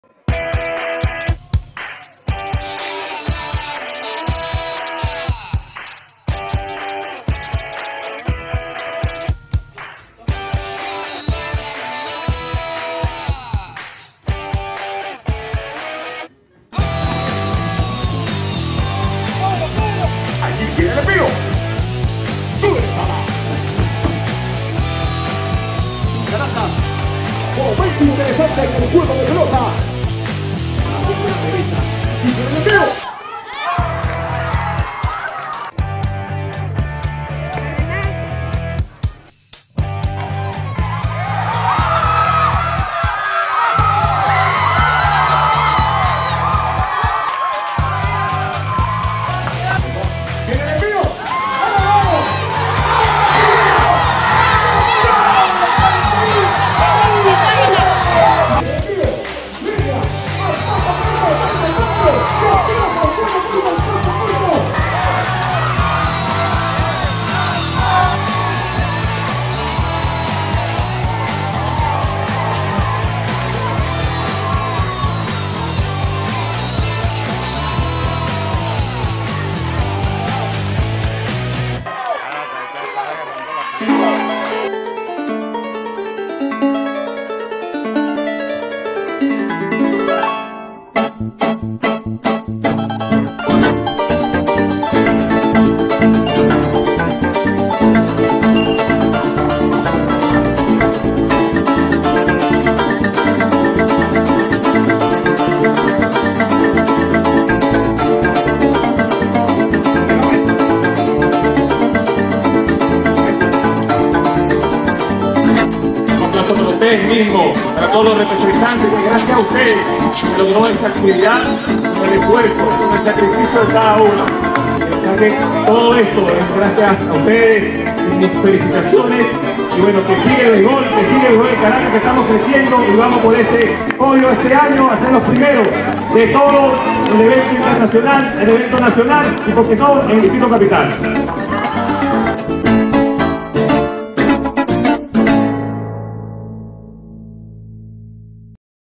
RESUMEN DISTRITALES CARACAS-VENEZUELA
Los Distritales Copa Chino Aponte 2023 llega a su final, donde se pudo apreciar un ambiente lleno de emociones con las 3 finales; en la Categoría Infantil A logró quedarse con el título el Team Caracas, en Formación Team Capital y en Iniciación Team Distrito; los MVP fueron: ISAAC VALESILLO (INICIACION), BEIKER PRIETO (FORMACIÓN) Y JONAS MARTINEZ (INFANTIL A), donde recibieron su obsequio por parte de @buhosports , luego en la clausura se inició con un baile Venezolano por parte de @academia_yenedance para darle inicio a las entrega de reconocimiento a los diferentes Atletas de Distrito Capital que representaron internacionalmente al País en el 2022 , a continuación entregaron los premios especiales: Atleta Femenino del Año; Mari Rodriguez, Atletas Masculino del Año: Aaron Arroyo, Tecnico del Año: Alejandro Moreno, delegada del año, Emmy Martínez y liga del Año Limenor.
“Quiero agradecerle primeramente a Dios a cada representantes, Atletas, staff y personal que apoyaron esta actividad estuvimos con grandes profesionales con la narración de
Henry el tata Hernandez con un excelente trabajo, Audio Digital, en el Sonido gracias a @gdcdeporte , @tu_obsequiofavorite con los certificados, @thebuhos1128 en los premios, @yenedance con los bailes y en el video @zona_strike1 ; a demás agradecerle a los cordinadores de Anotador Belkys Samaro y de los Arbitros Romer Diaz. “ Fueron las palabras Niowaldo Zorrilla Presidente de la Asociación de Béisbol del Distrito Capital.

Gracias a nuestro aliado: @oliver.films